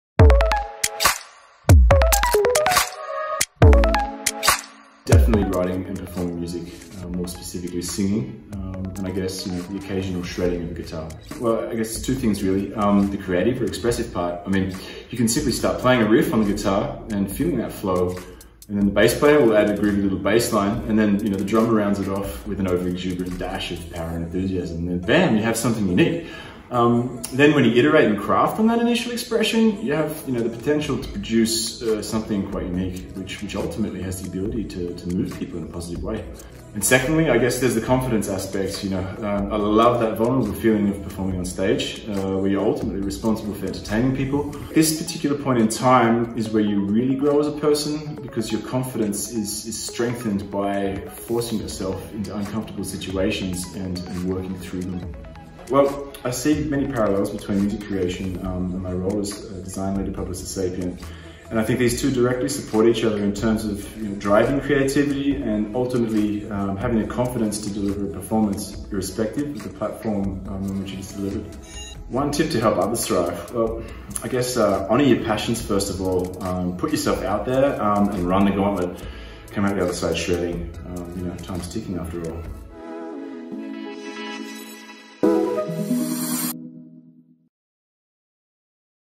Nicholas' How I Thrive Story
Nicholas, Design Lead and Musician, discusses how his passion for writing and performing music drives creativity and builds confidence in his daily life.